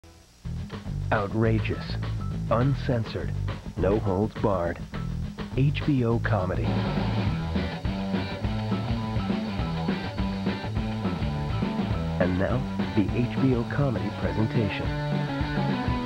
HBO Comedy ID (2005)
Submission sent by Smok.

Source: Seen before an August 9th, 2005 airing to The Kathy & Mo Show: Parallel Lives.